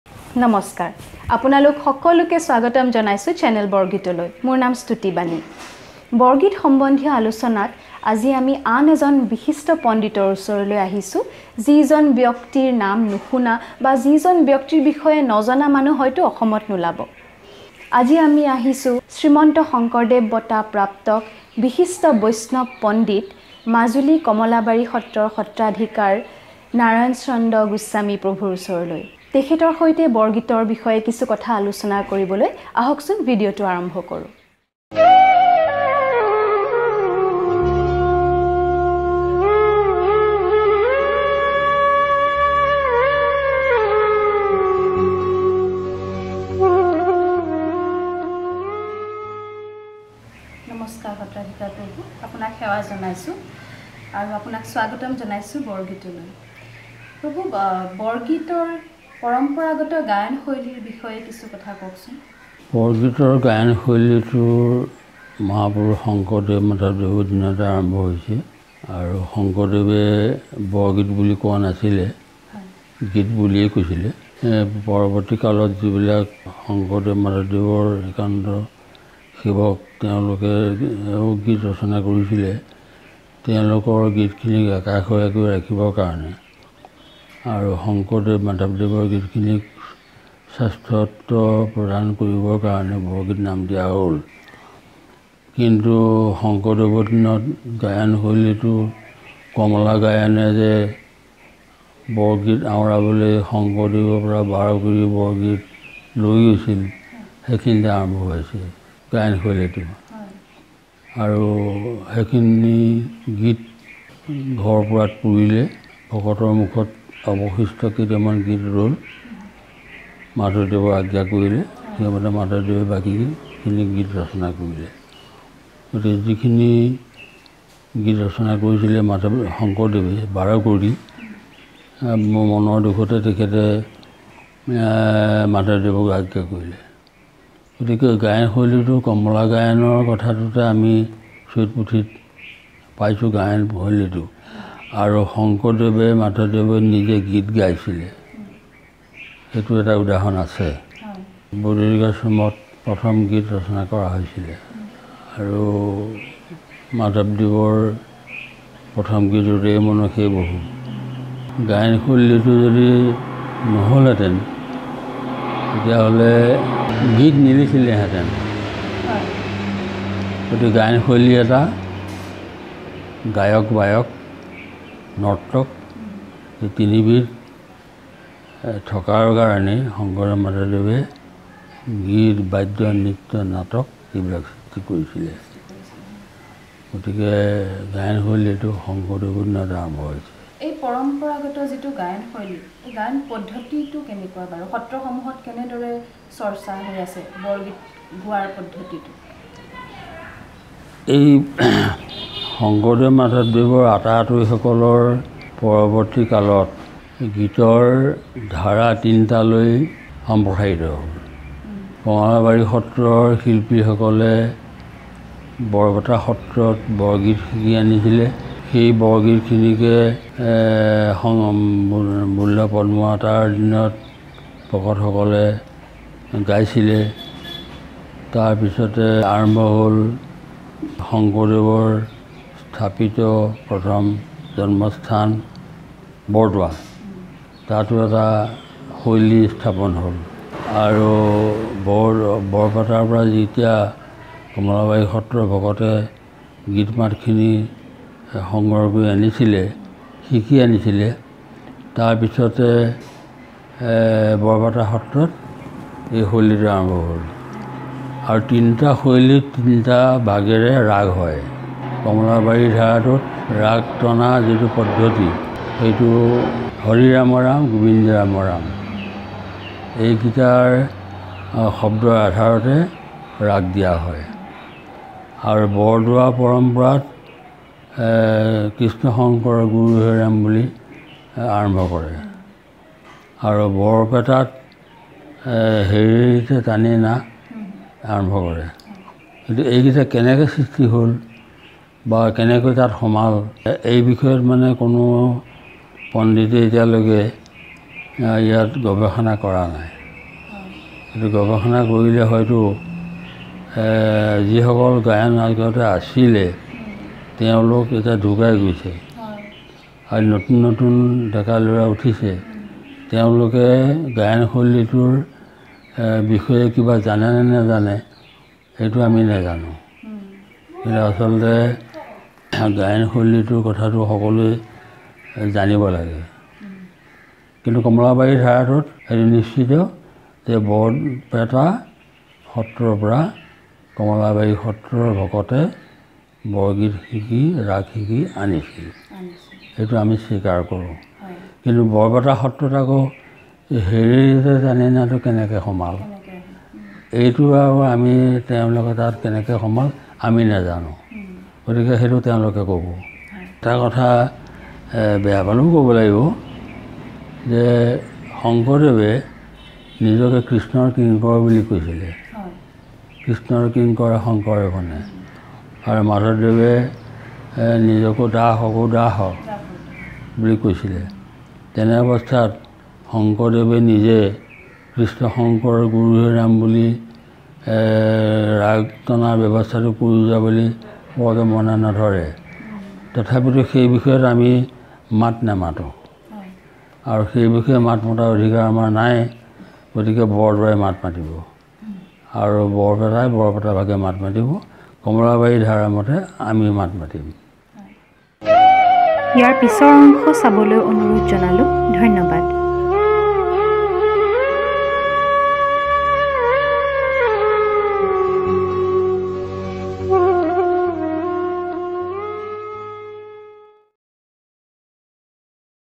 Discussions with Narayan Chandra Goswami ( part -1 ) | by Stuti Bani
অলপতে আমি সত্রনগৰী মাজুলী দৰ্শনৰ বাবে গৈছিলো । মাজুলীৰ ইতিহাস প্ৰসিদ্ধ বিভিন্ন  সত্র দৰ্শন কৰাৰ ওপৰিও শ্ৰী শ্ৰী শংকৰদেৱ বটা বিজয়ী মাজুলী কমলাবাৰী সত্ৰৰ সত্ৰাধিকাৰ  শ্ৰীযুত নাৰায়ণ চন্দ্ৰ গোস্বামী প্ৰভূক সাক্ষাত কৰি তেখেতৰ লগত বৰগীত সন্দৰ্ভত কিছু আলোচনা কৰা হৈছিল । সেই আলোচনাৰ কিয়দংশ আপোনালোকৰ বাবে এই ভিডিঅটি যোগেদি আগবঢ়াইছো ।

Special thanks : Vivek Kalita